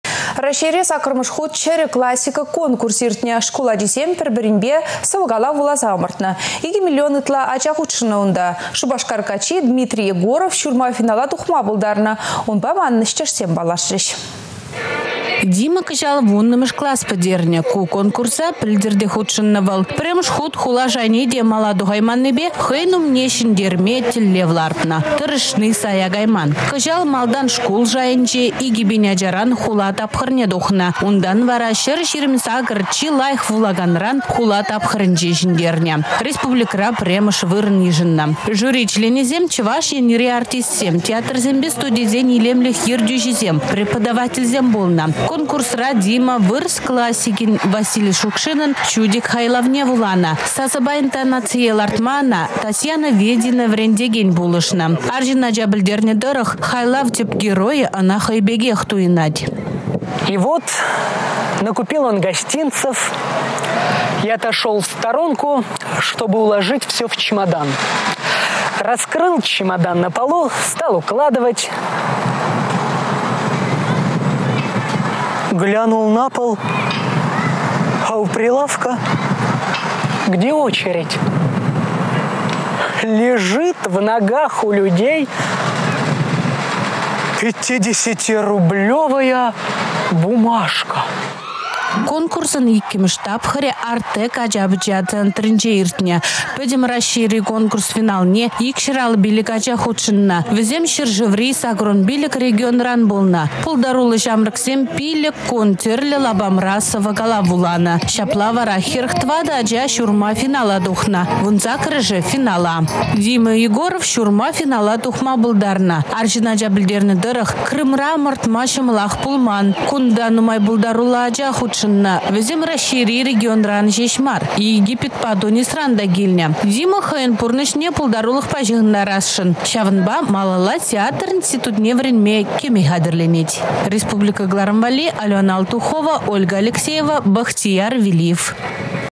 «Чĕрĕ классика» конкурсăн республика çĕнтерӳçи
Раççейре саккăрмĕш хут «Чĕрĕ классика»  конкурс иртнĕ. Шкул ачисем пĕр-пĕринпе сăвă-калав вуласа ăмăртнă. Икĕ миллион ытла ача хутшăннă унта. Шупашкар каччи Дмитрий Егоров çурма финала тухма пултарнă. Унпа манăн ĕçтешсем паллашрĕç.